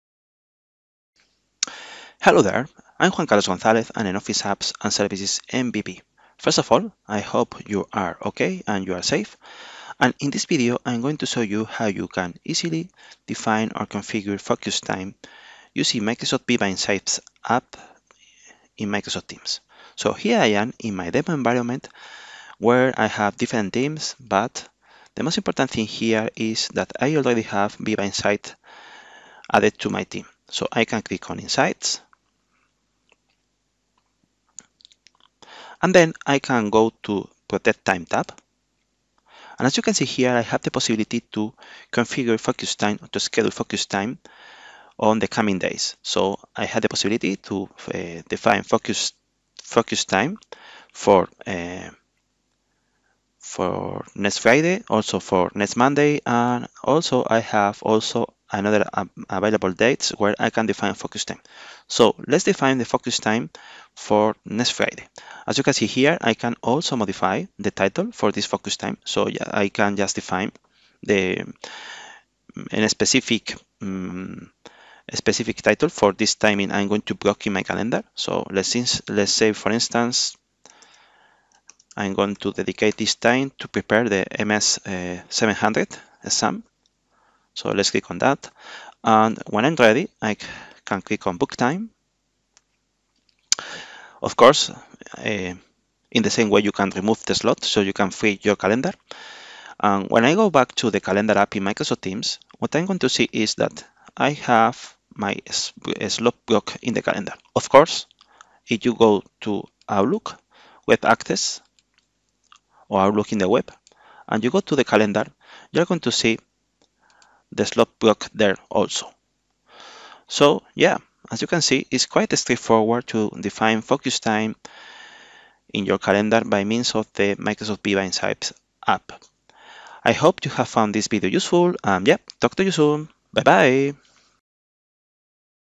Microsoft 365 - Scheduling focus time in Microsoft Teams with Viva Insights
In this video I show how to schedule focus time in Microsoft Teams with Viva Insights.